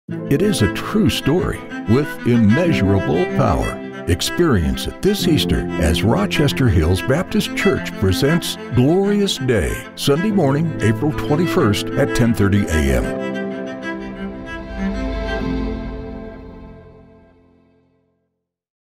RHBC - Glorious Day 2019
Join us as Rochester Hills Baptist Church Presents “Glorious Day,” a captivating narrative of the life and death of Jesus Christ. for more info!